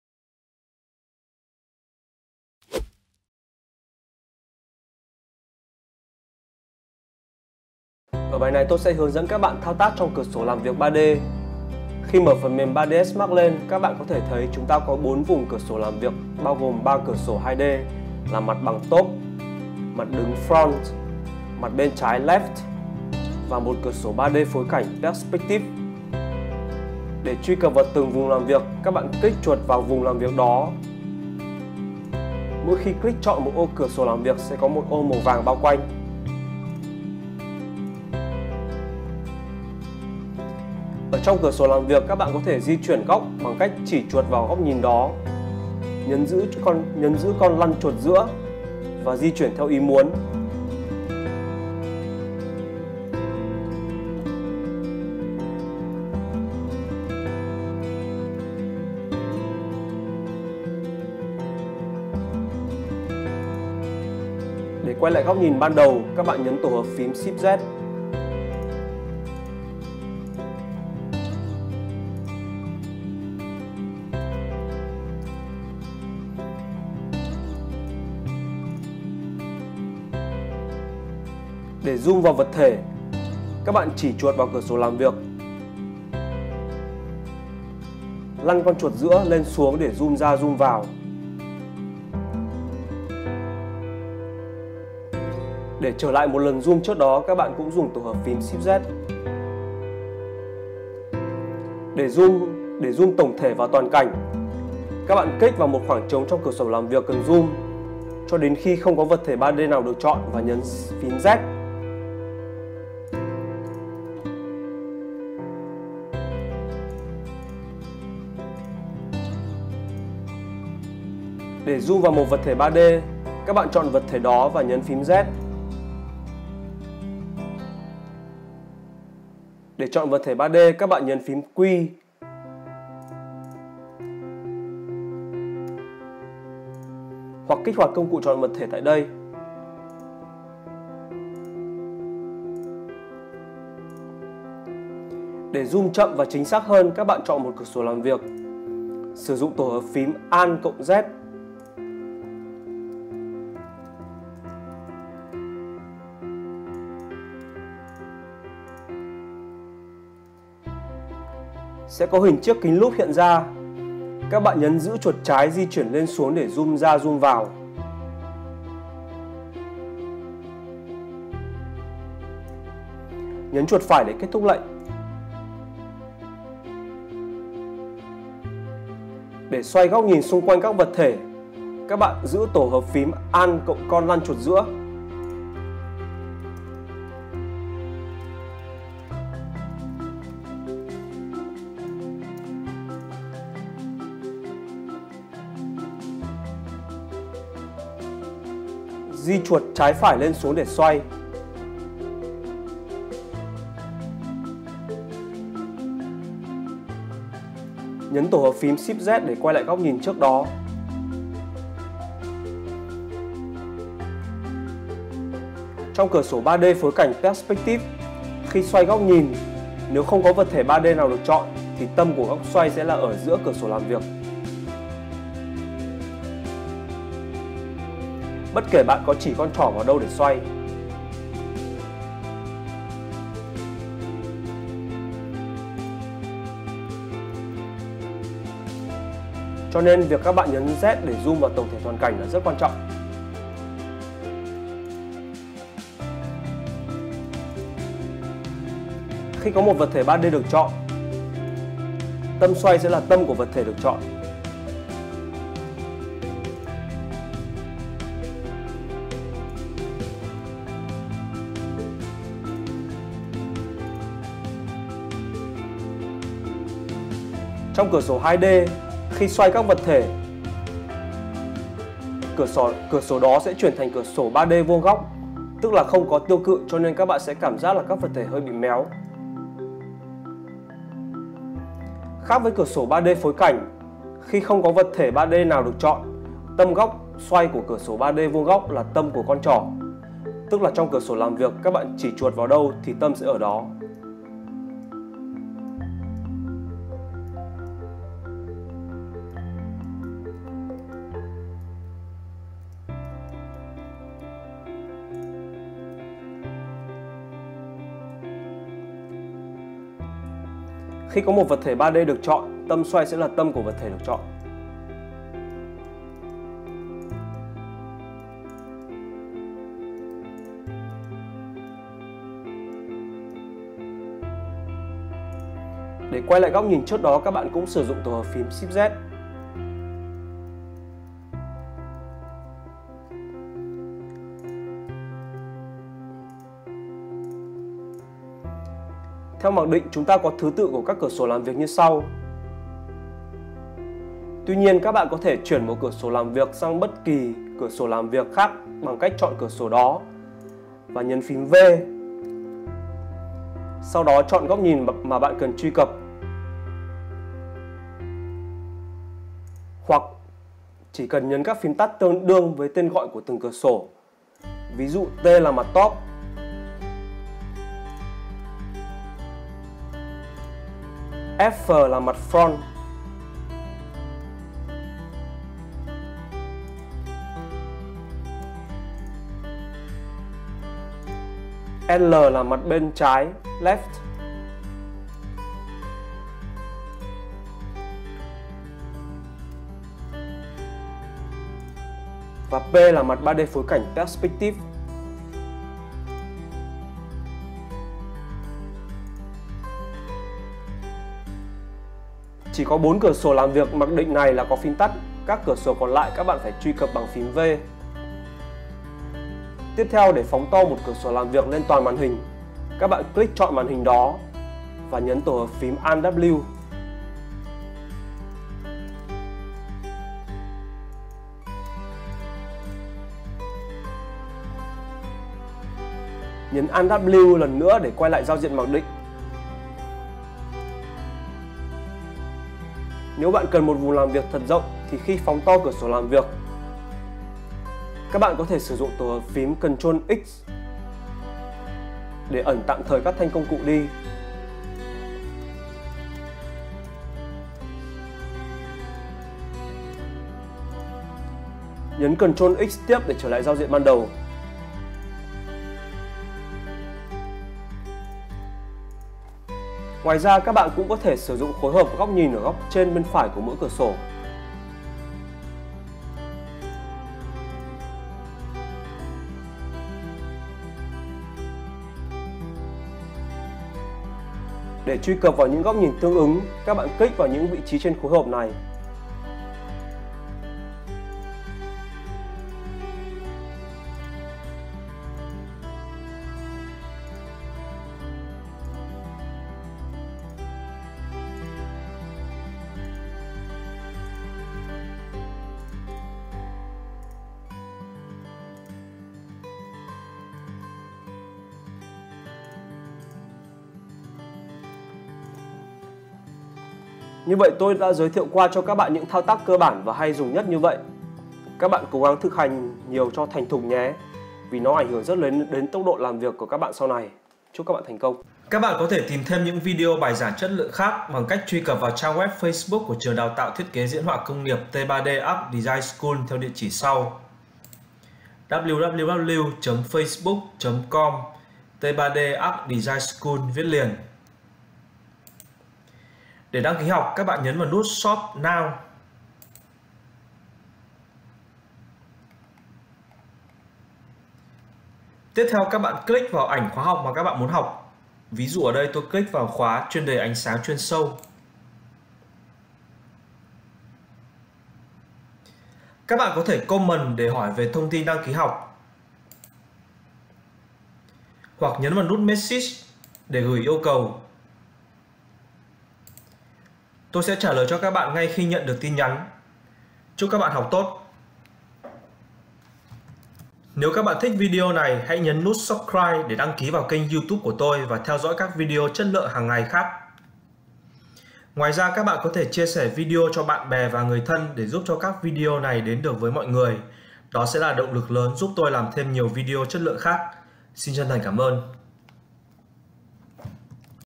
Thao tác cơ bản trong cửa sổ làm việc 3ds max - Basic Navigation Inside 3ds Max
1. Mô tả
2. Description
3. Links
1. Mô tả:
Bài học này sẽ hướng dẫn các bạn thao tác cơ bản trong cửa sổ làm việc của phần mềm 3ds max

2. Description:
This video show you how navigate inside 3ds max window workspace.

3. Links:
T3D Youtube Channel: